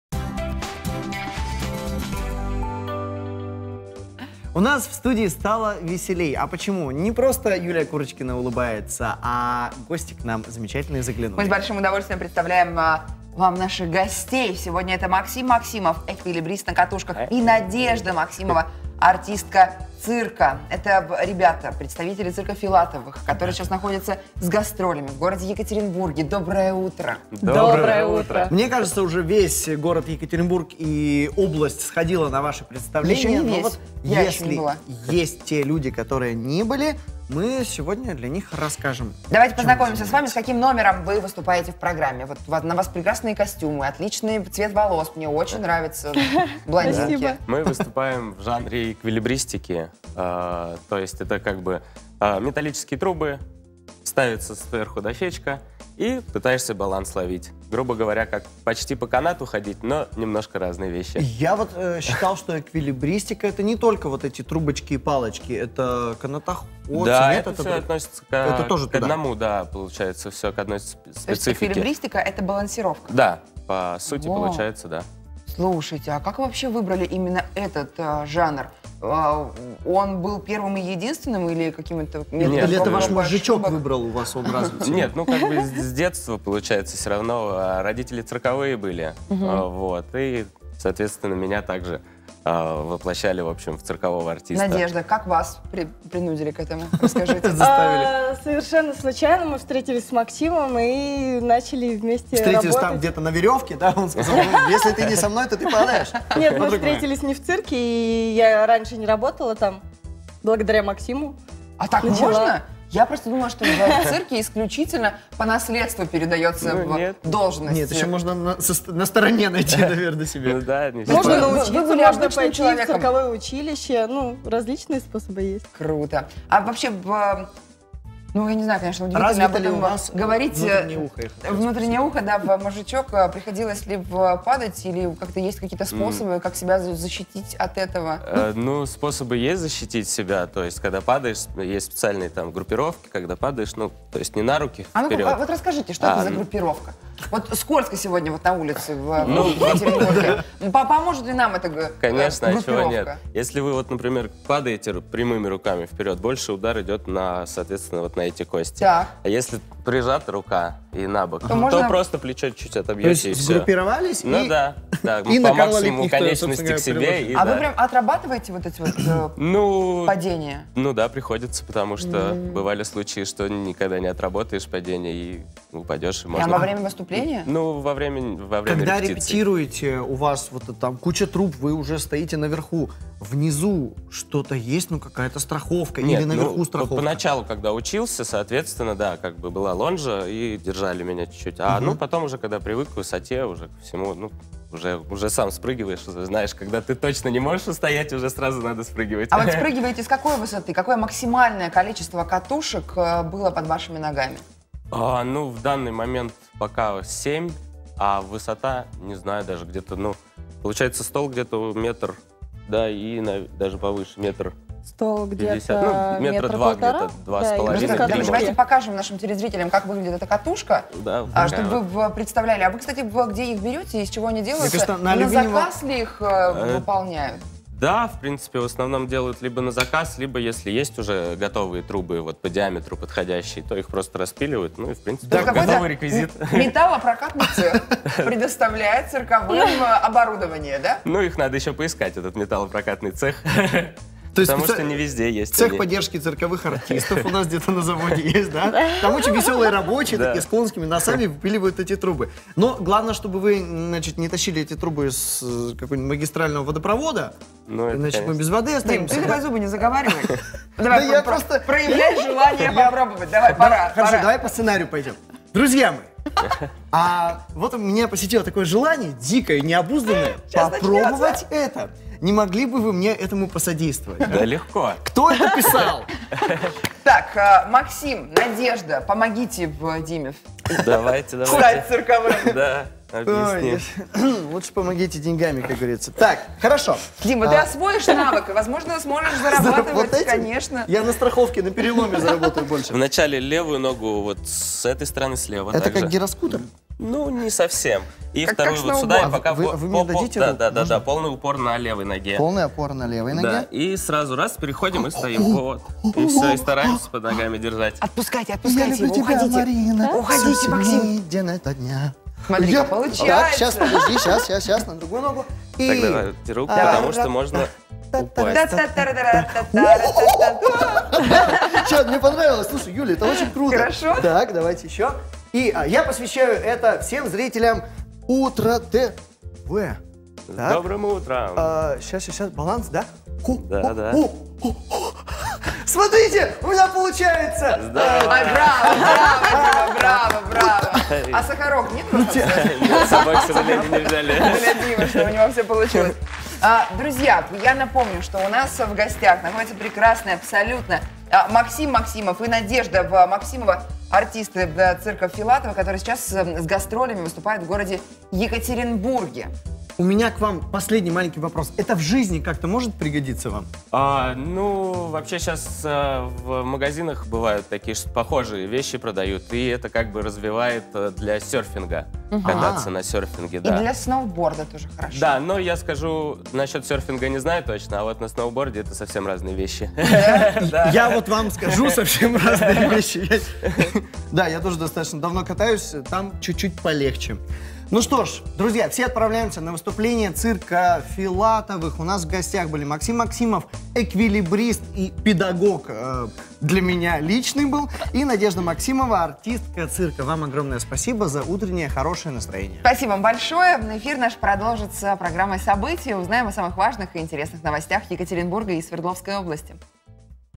Максим и Надежда Максимовы — артисты цирка Филатовых
Мы продолжаем знакомить вас с артистами всемирно известного цирка Филатовых. Сегодня к нам в студию пришли представители ещё одной цирковой династии, невероятные Максим и Надежда Максимовы!